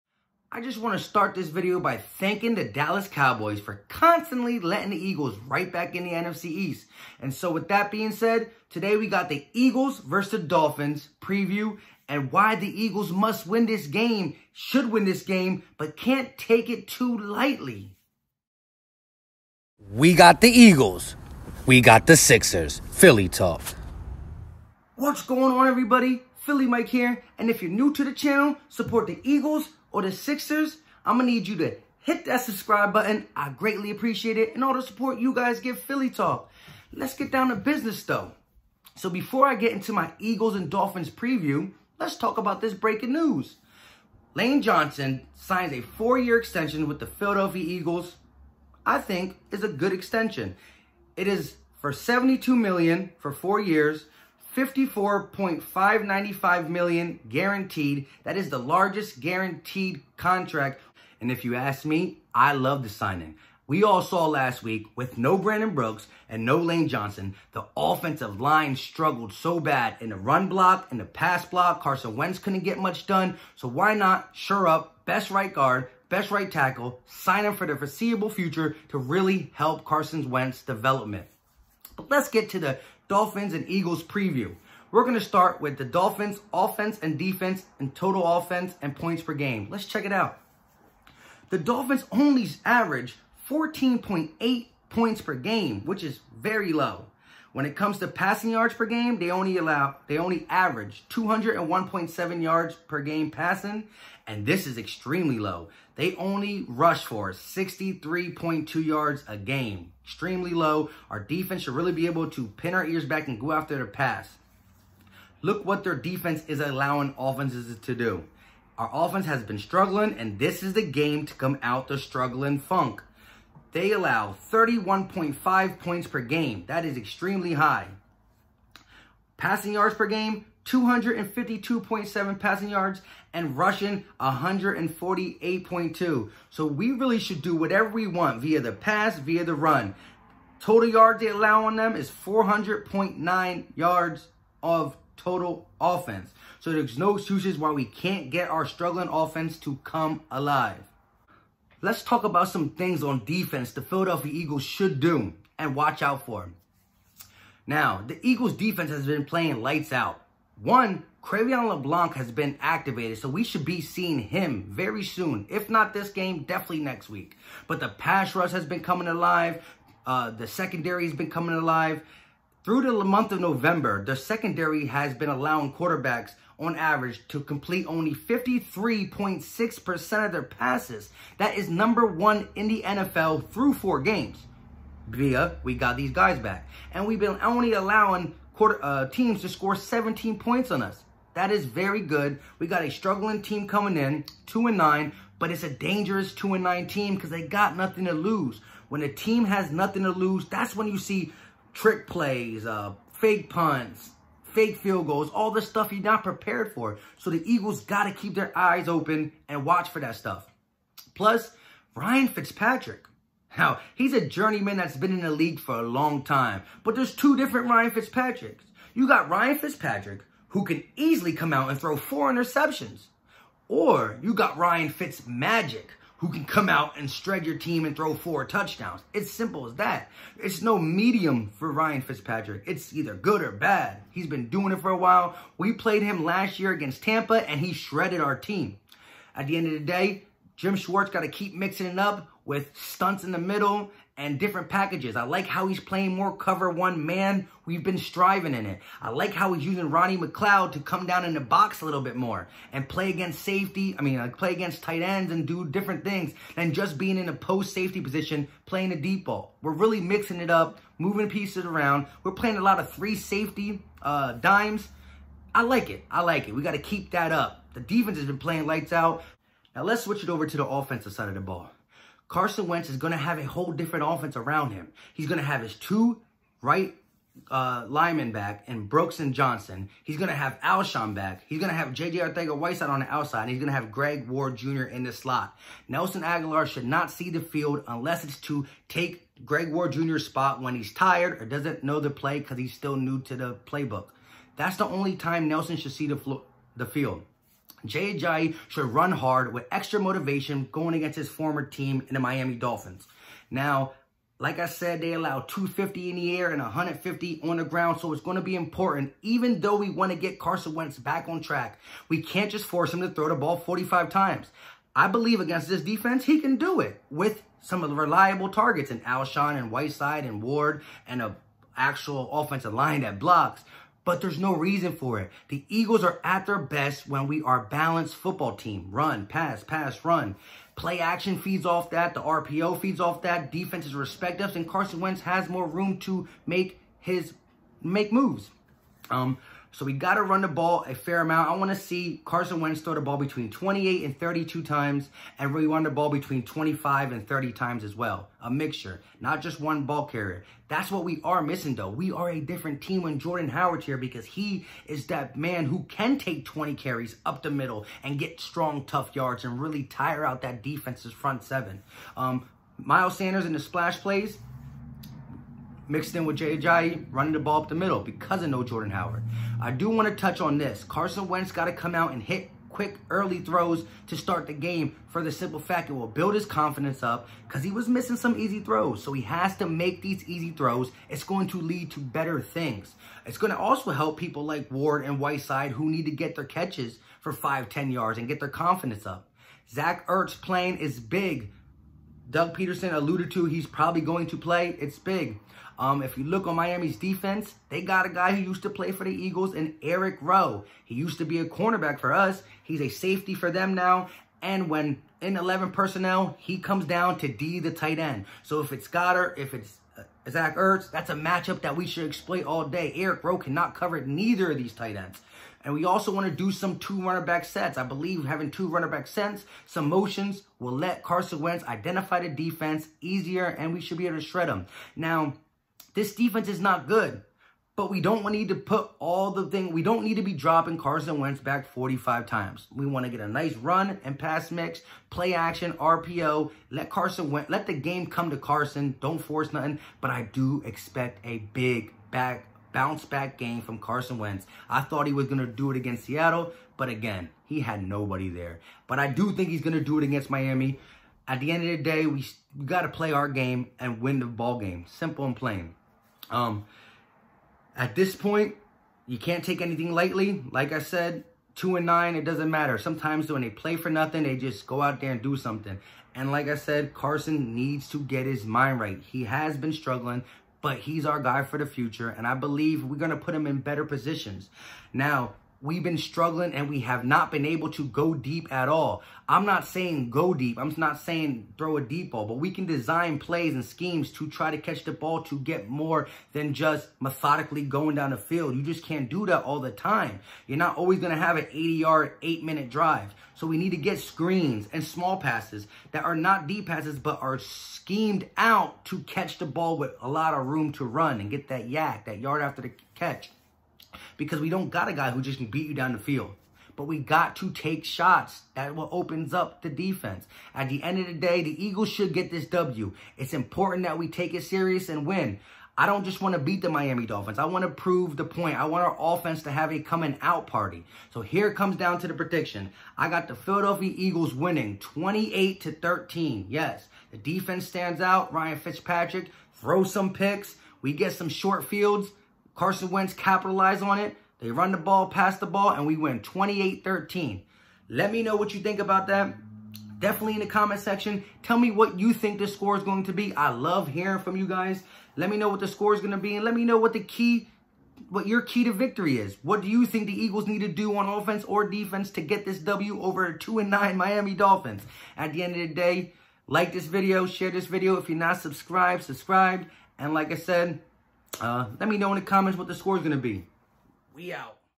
Philadelphia Eagles vs Miami Dolphins Preview| Lane Johnson Extension| Must win game
The Philadelphia Eagles vs The Miami Dolphins and a much needed win for the Eagles. Eagles defense has been lights out in the offense turning around to help the defense? The Eagles control their own destiny for the playoffs. Will they get to the postseason?